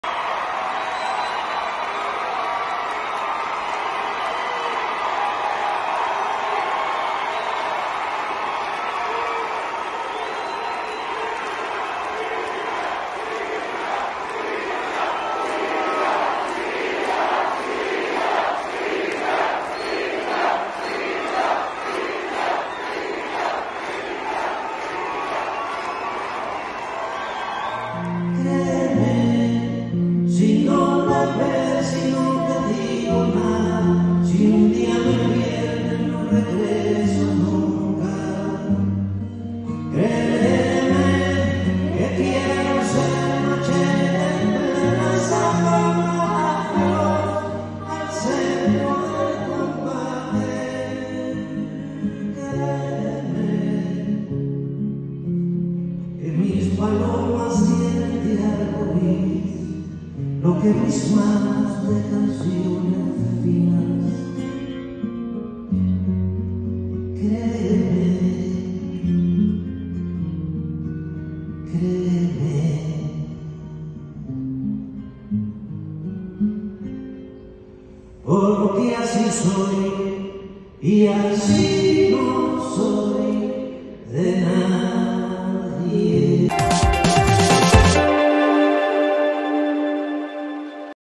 Silvio Rodríguez vuelve a cantar en México ocho años después
¡Viva México! ¡Viva Cuba! se oyó entre los miles de asistentes.